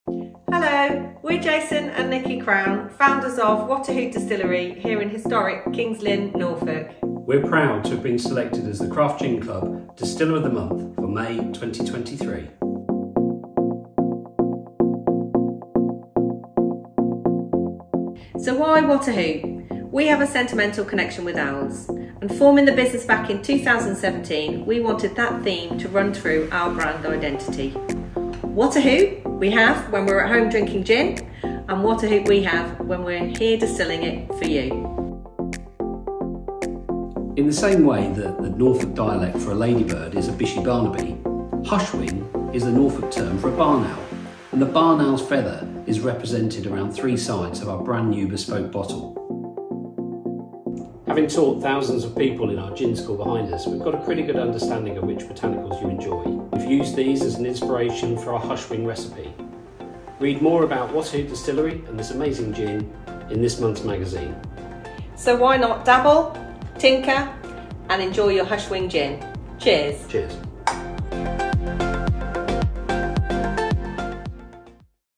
Meet the Maker: WhataHoot Distillery (WhataHoot Hushwing Gin)
Get an exclusive look inside one of the best craft gin distilleries in the UK: WhataHoot!

This Norfolk-based gin distillery is the amazing company behind WhataHoot Hushwing Gin, Craft Gin Club's May 2023 Gin of the Month, and its wonderful founders are here to tell you their story.

Gin fans, you don't want to miss this incredible gin tour!

Find out more about WhataHoot distillery and their gins right